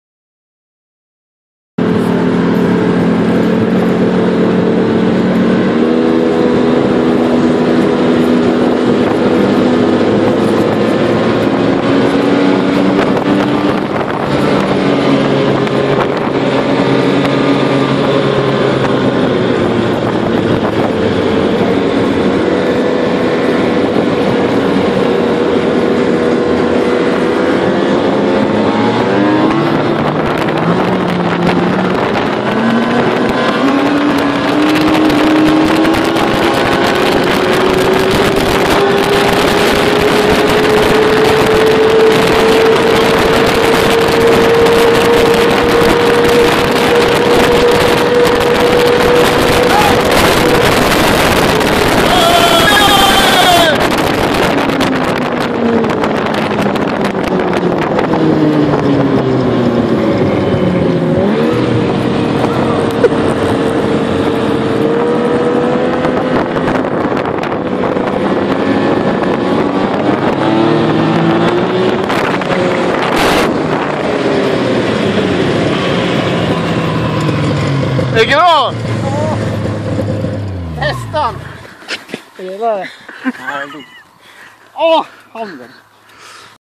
Boat crash on frozen lake
Unfortunately was not the accident on film :)
I'll stick to motorcycles in the future, I was quite bruised after the crash